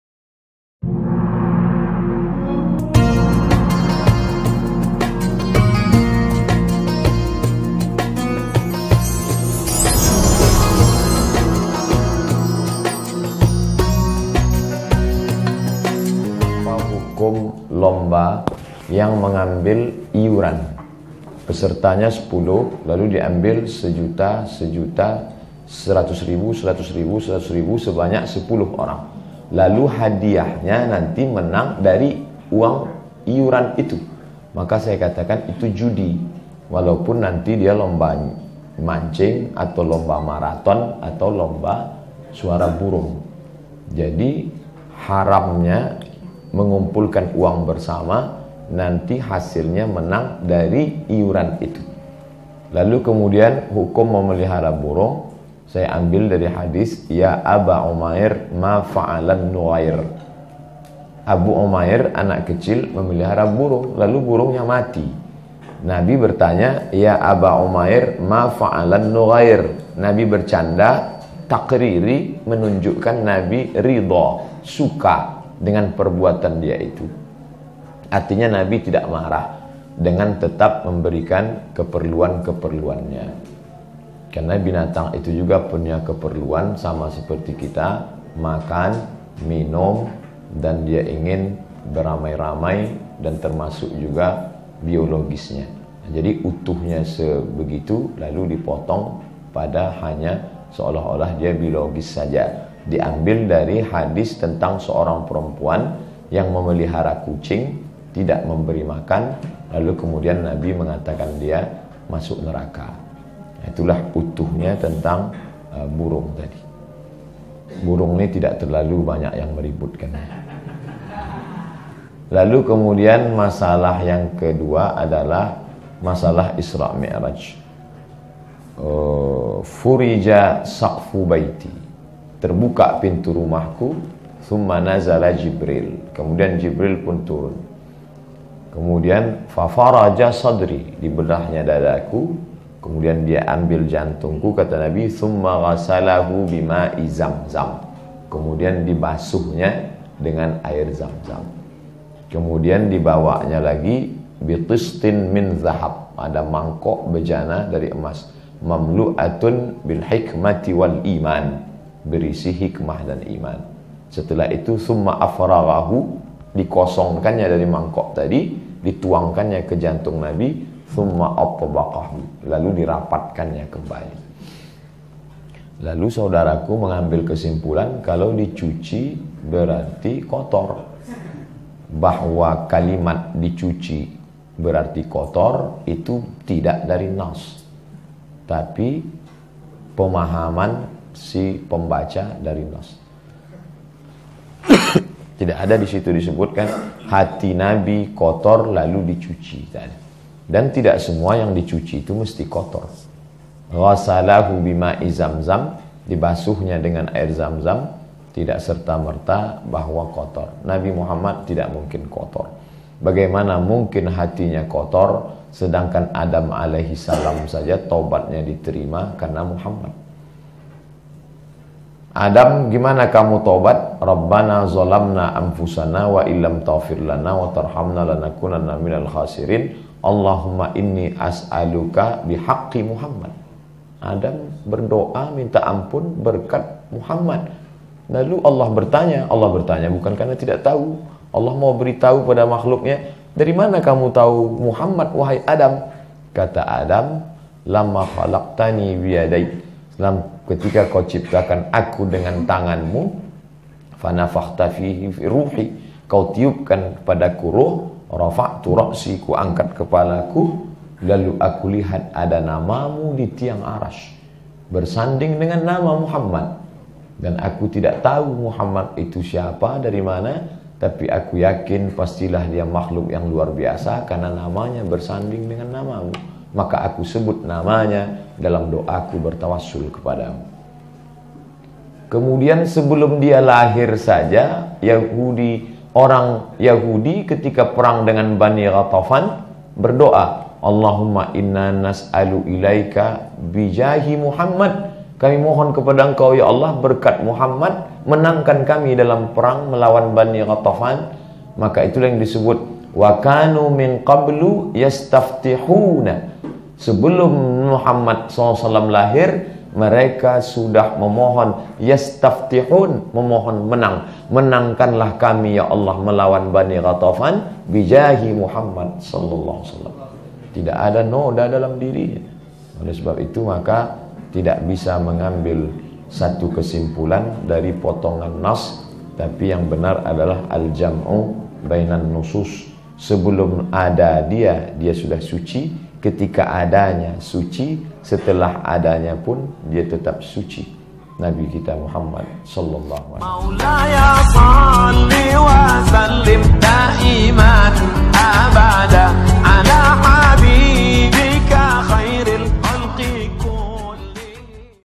HUKUM MEMBUAT LOMBA YANG MENGAMBIL UANG IURAN | Ceramah Ustadz Abdul Somad
KONTEN DAKWAH ISLAMI

Video Sharing Ceramah Dari Kami Untuk Membuat kita semua lebih baik kedepannya Amiinnn...

Sampaikan Walaupun Satu ayat.

Terima Kasih. Wassalam🙏